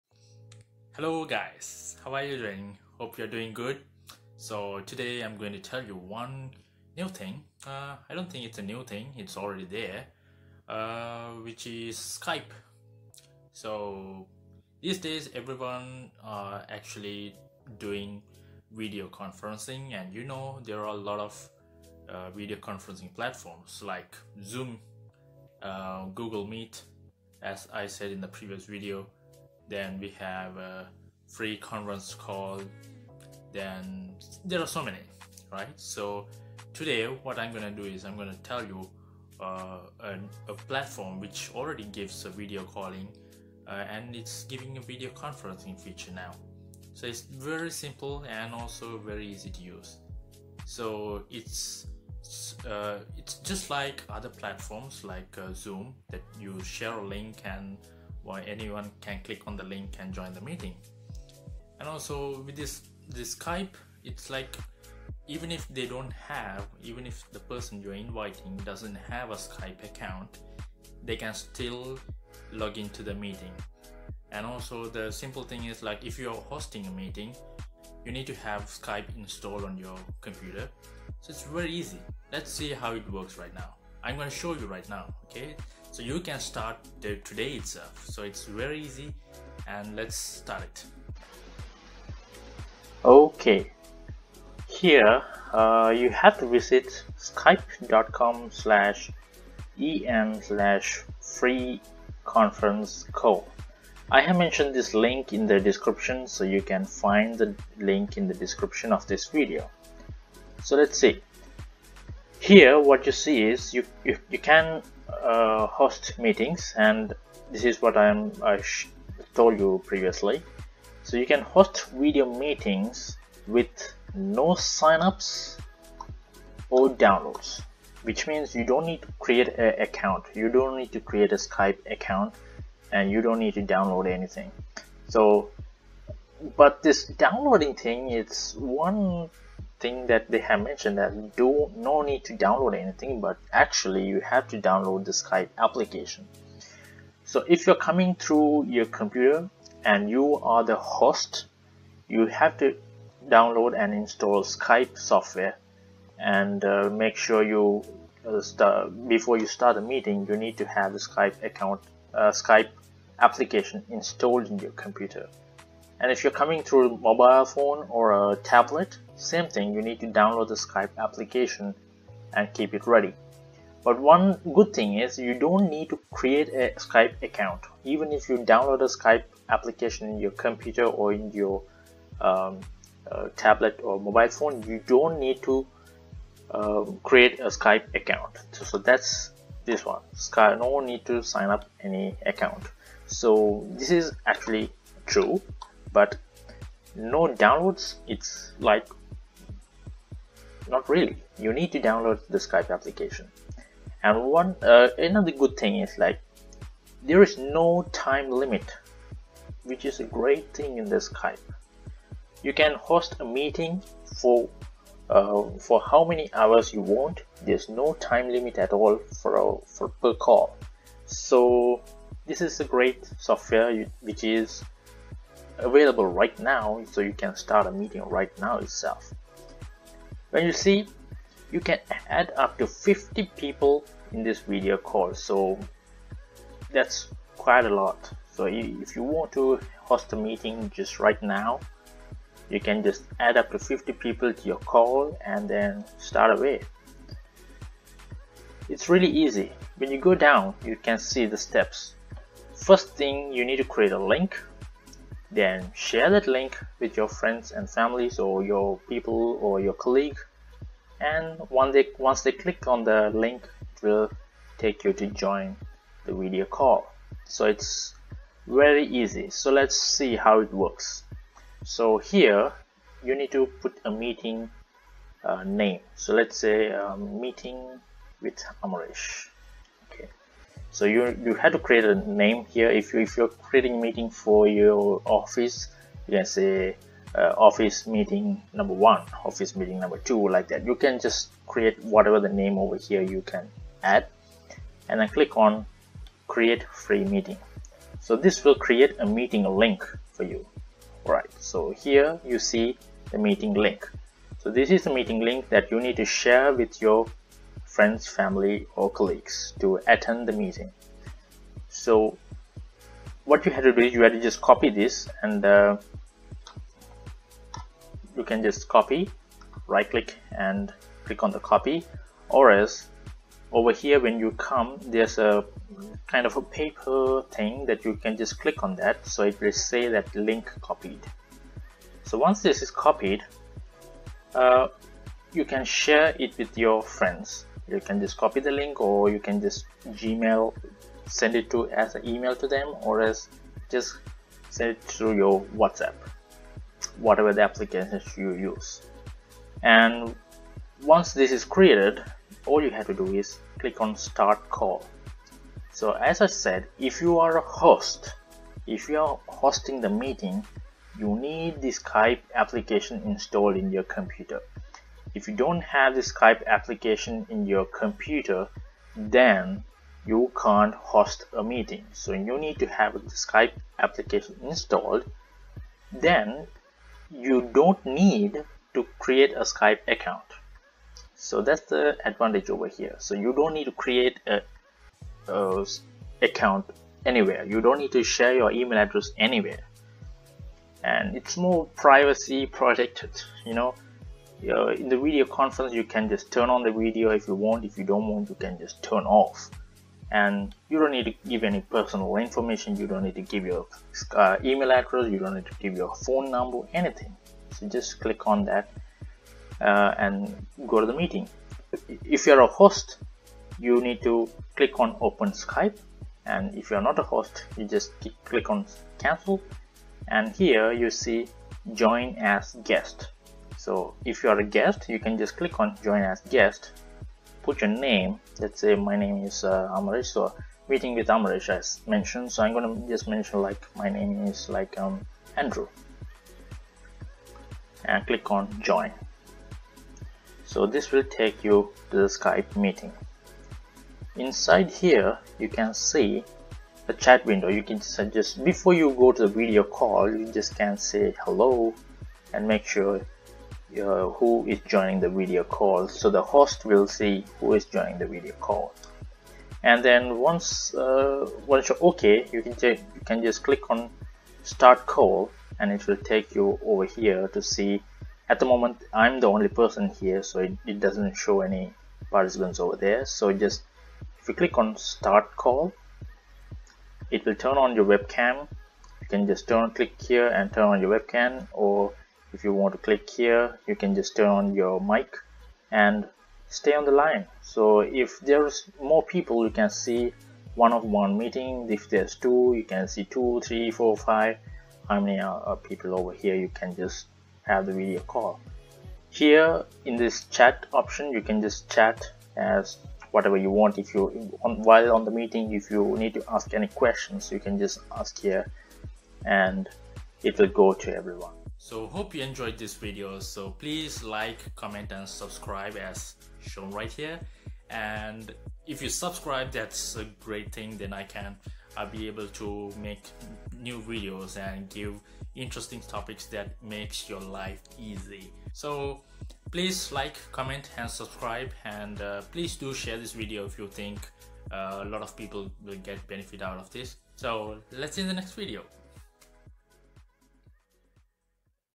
How to host a FREE VIDEO MEETING with no sign ups or downloads using Skype!
Skype has launched it Meet Now video conferencing feature to compete with Zoom and other video conferencing platforms out there. During this COVID-19 everyone started to work from home and have meetings via online. So Skype decided to compete with the recently become popular video conferencing platform Zoom.

It's easy and free! - simply create a meeting link, share the link with participants and join the call. No sign ups required. Check this video till the end. I am showing you how to create a meeting instantly and start a meeting right away.

Based on my opinion, this is way easy for anyone to start a video conferencing right away for free. And when you can have upto 50 participants can attend a video conferencing, that's an amazing feature ever! Specially you don't need to create a Skype Account or even share any of your personal information.